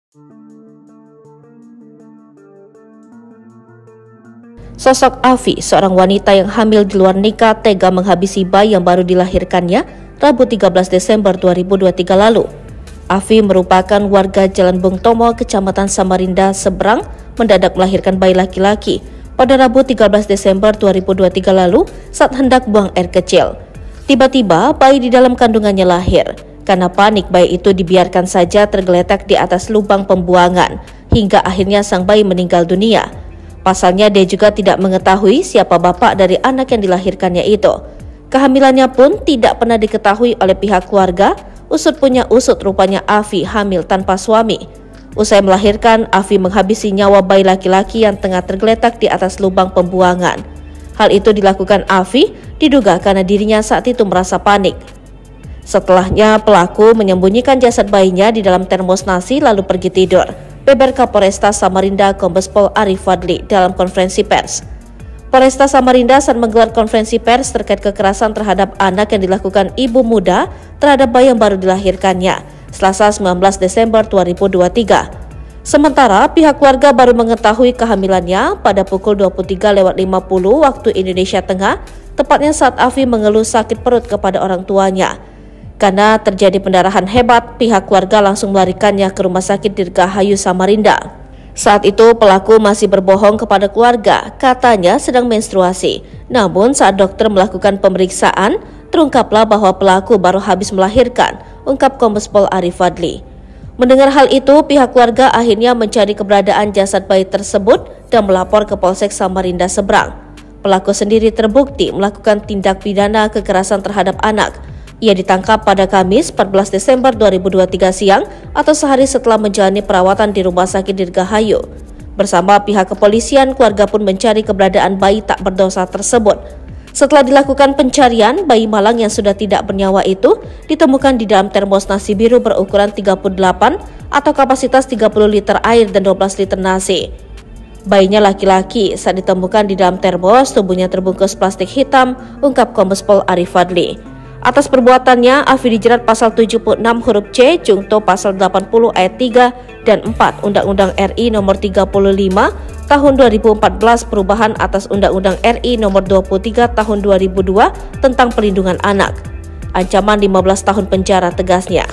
Panik Melahirkan Tapi Tak Tau Siapa Ayah Anaknya, Ibu Muda di Samarinda Lakukan Hal Tak Patut
Sosok AVI (22), seorang wanita lakukan hal tak patut pada bayi yang baru dilahirkannya, Rabu (13/12/2023) lalu.

AVI merupakan warga Jalan Bung Tomo, Kecamatan Samarinda Seberang mendadak melahirkan bayi laki-laki pada Rabu (13/12/2023) lalu saat hendak buang air kecil.

Tiba-tiba bayi di dalam kandungannya lahir.

Lihat informasi terkini bersama kami, di:

Palembang.tribunnews.com

Sumber Video: kolase foto tribun
Editor Video: reecha
vo: ulid